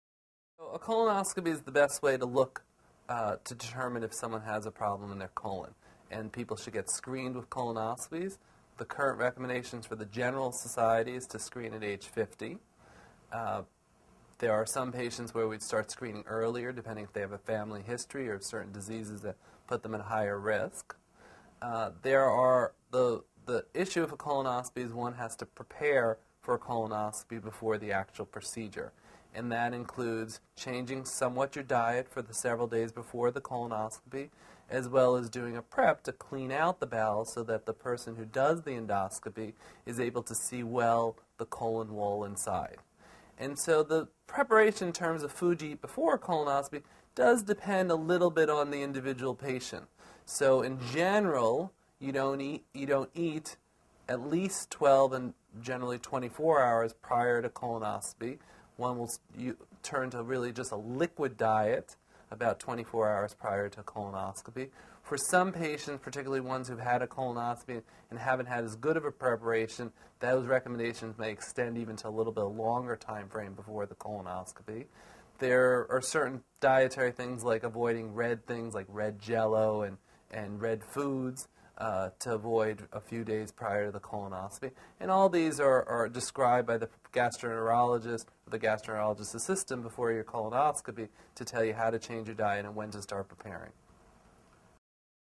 What to Eat Before a Colonoscopy
What to Eat Before a Colonoscopy. Part of the series: Colon Cancer Facts. Before a colonoscopy, people generally are told to switch to a liquid diet 24 hours before and to avoid red-colored foods several days before. Prepare for a colonoscopy for the most accurate results with helpful information from an oncology specialist and assistant professor of medicine in this free video on cancer.